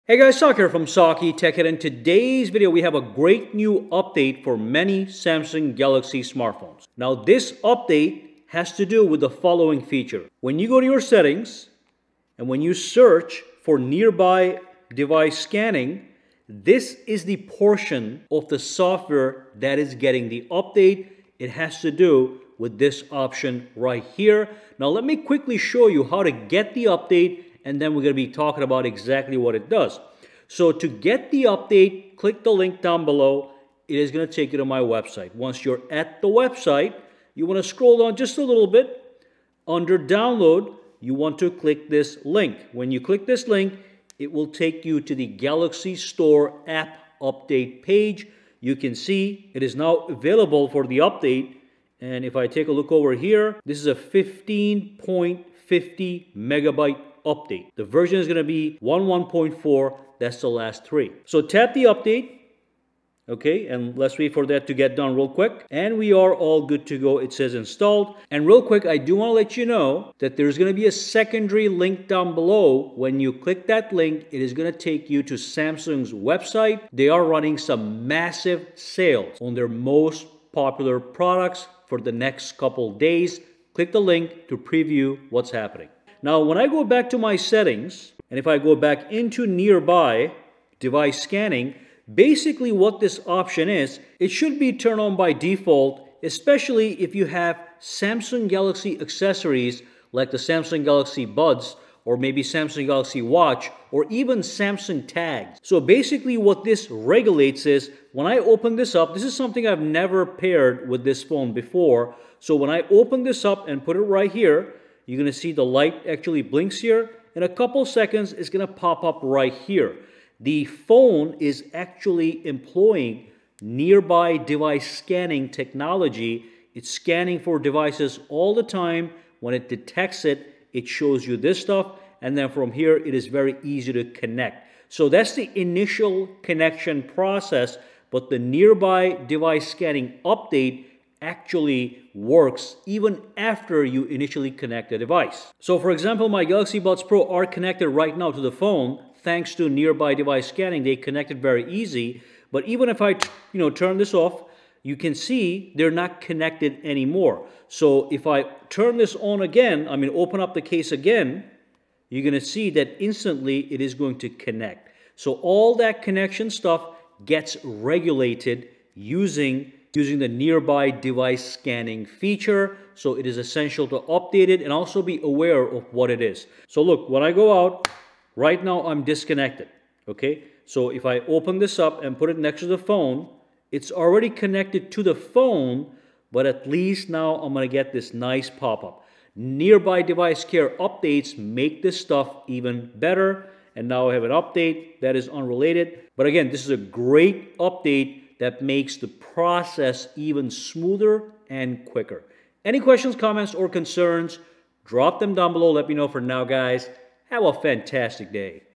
Great New Samsung Update for All Samsung Galaxy Smartphones! - What's New?
There is a great new Samsung update that makes a critical feature much better. Let's dive in and see what's new.

Update is available for many Samsung Galaxy smartphones such as Galaxy S23 Ultra, S22 Ultra, S21 Ultra, Note 20 Ultra, Fold 3, Flip 3 and more.

Applies to One UI 5.0, 4.1, 4.0, 3.0, etc

Companies can send product to the following address:

Attn: SAKITECH
135 S Springfield Rd. Unit 681